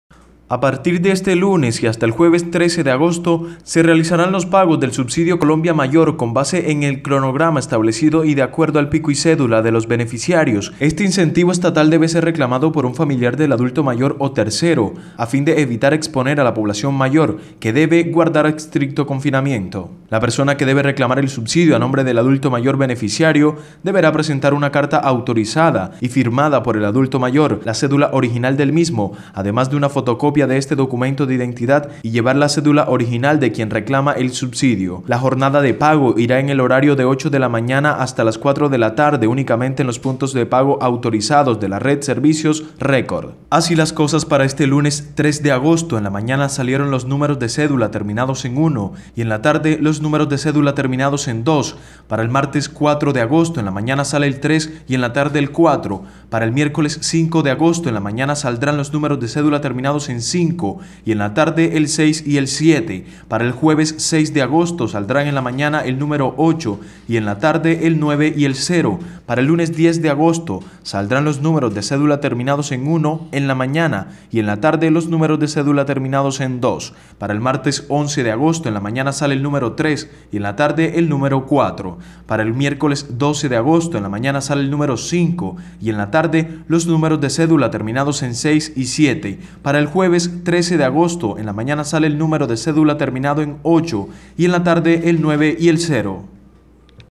Este es el cronograma de pagos del subsidio a los adultos mayores en Montería
La jornada de pago irá en el horario de 8:00 de la mañana hasta las 4:00 de la tarde.

¡Siempre Informando!